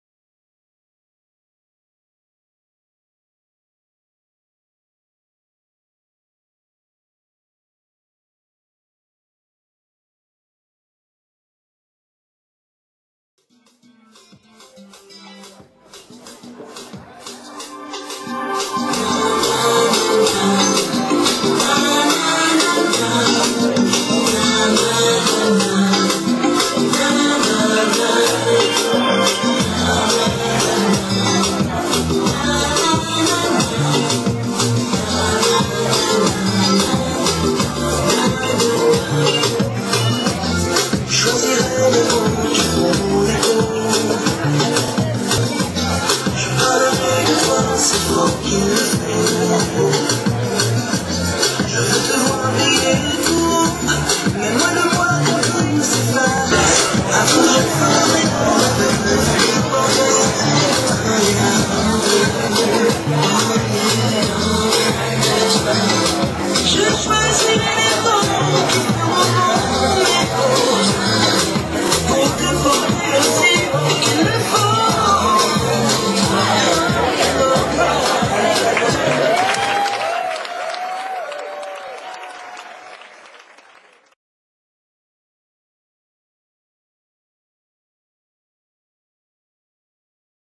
Zouk Performance by Apple and Tropical NY @ United Nations Party (New York, USA)
Apple and Tropical NY performed Zouk at the International Night hosted by the United Nations @ the United Nations, New York, USA.